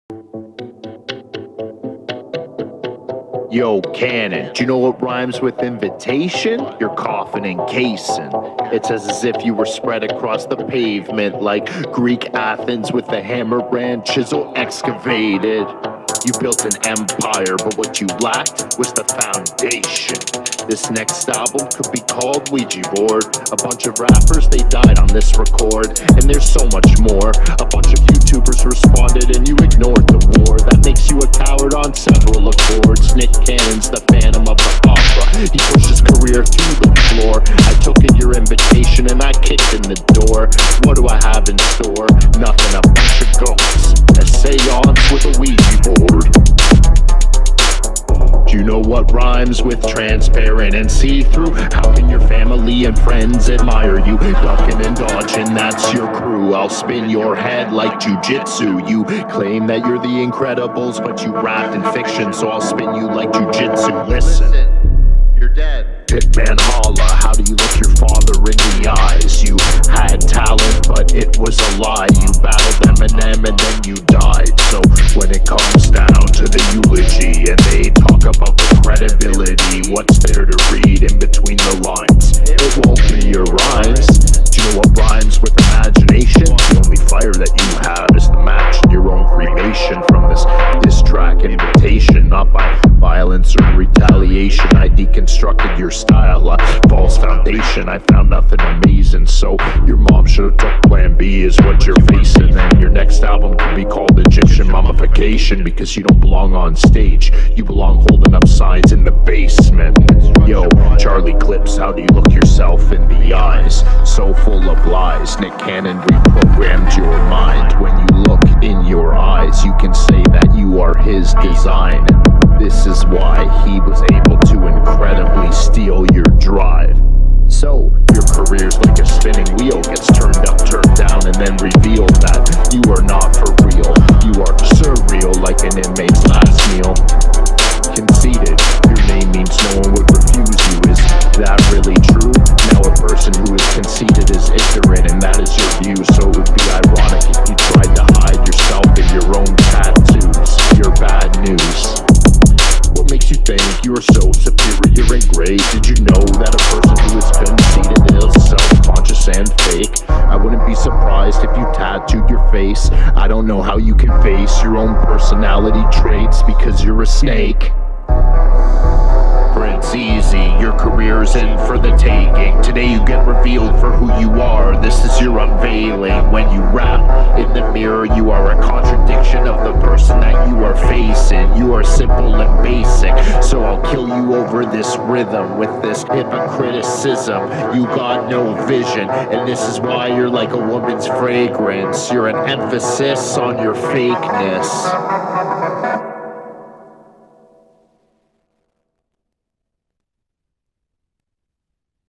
Nick Cannon Wild N Out Incredible Diss
When Nick Cannon and his crew dissed Eminem. I was unimpressed with the diss. I thought the rhymes were terrible. A lot of other people dropped diss tracks towards Nick Cannon and  I feel like my bars need to be heard even though this is coming out so much later. I am sorry I got busy and one thing led to another.